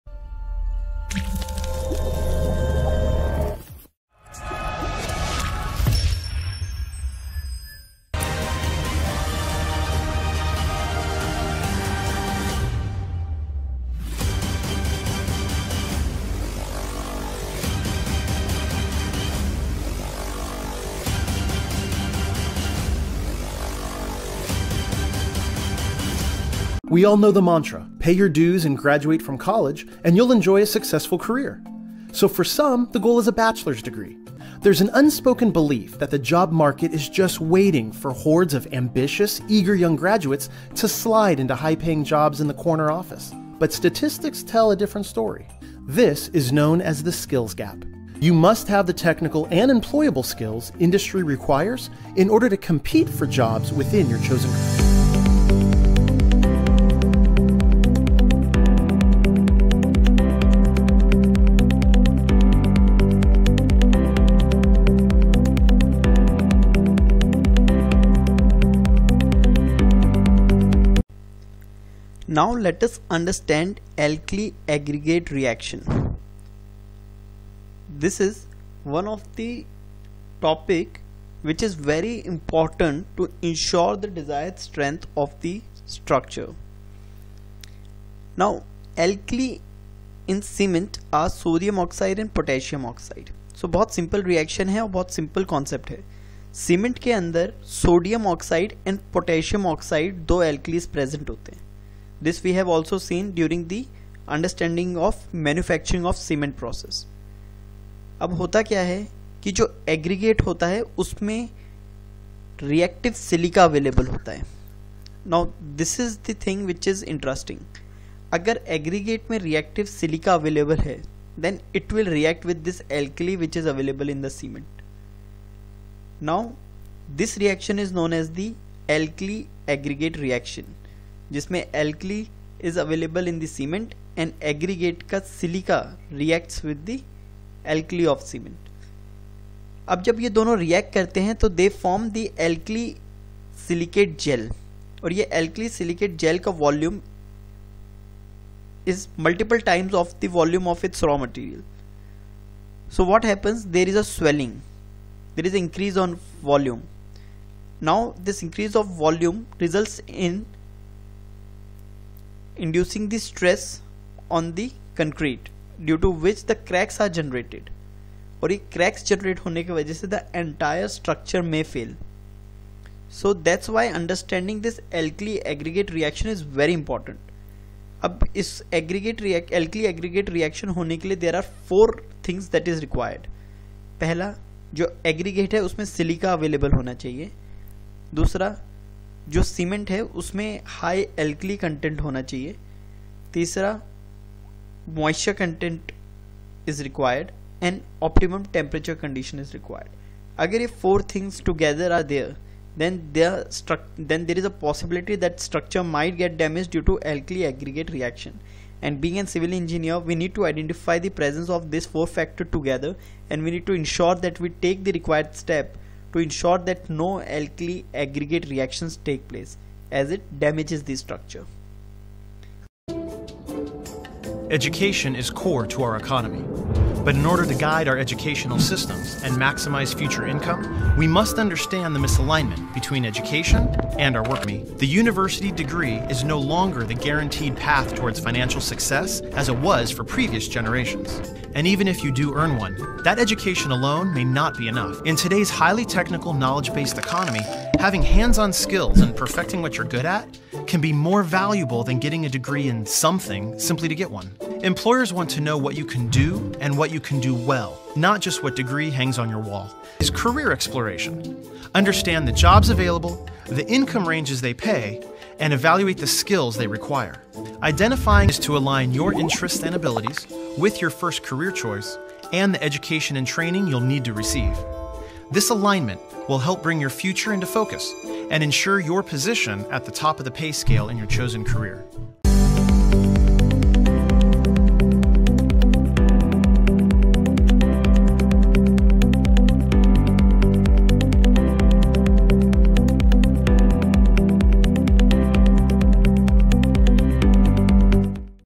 Aggregate Alkali Reaction in Aggregate CONCRETE TECHNOLOGY MODULE 1.3.8
8. Alkali Aggregate reaction
Alkalies in cement are sodium oxide and potassium oxide.

Some of the aggregate contain reactive silica, which reacts with alkalies present in cement i.e sodium oxide and potassium oxide.

When these reaction takes place, alkali silicate gel of unlimited swelling type are formed this starts generating cracks in concrete and may lead to ultimate failure of the structure.

Factor which promote Alkali Aggregate reactions are:

1. Reactive type of aggregate

2. High alkali content in cement

3. Availability of moisture

4. Optimum temperature conditions